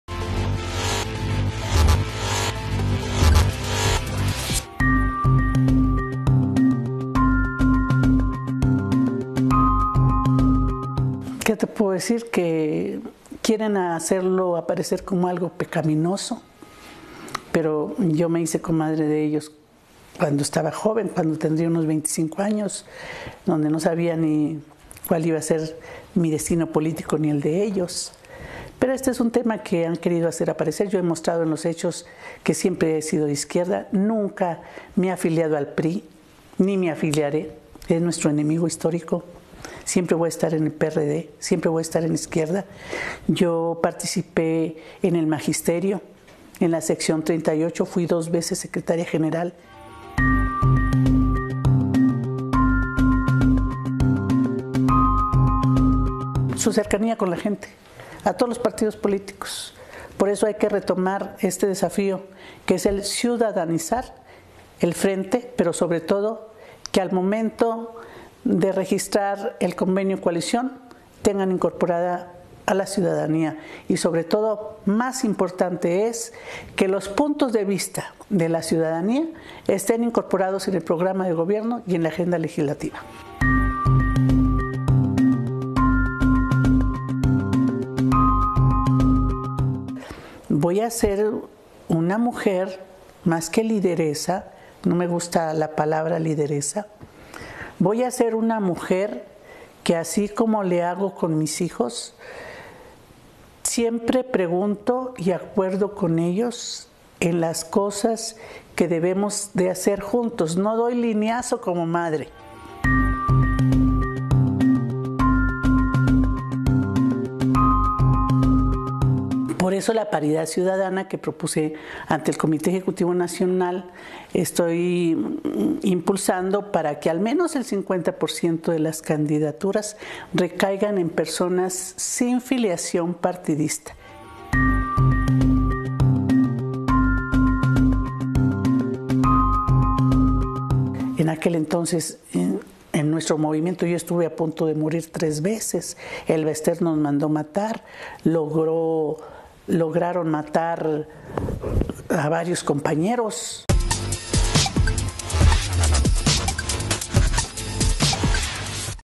“No es pecado ser comadre de Moreira”
Mary Telma Guajardo, secretaria de Alianzas del PRD, aspira a dirigir el PRD